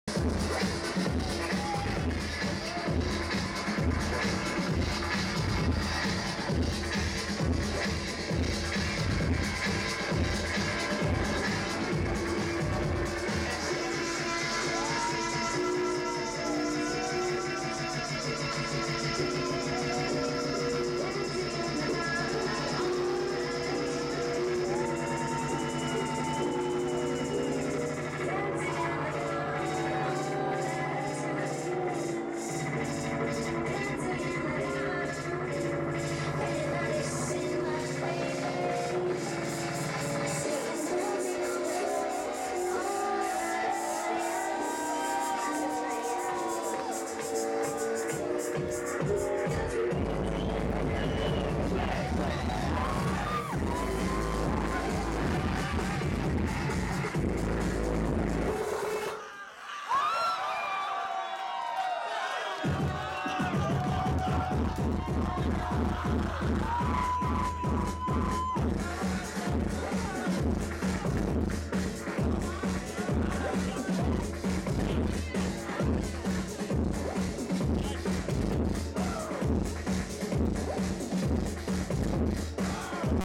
2o1o.o1.23-24 Atomic Hooligan at Fabrique (part 05 e01)
Atomic Hooligan Live at Fabrique, Minsk, Belarus.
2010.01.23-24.
shot in HD by SpAwN_gUy.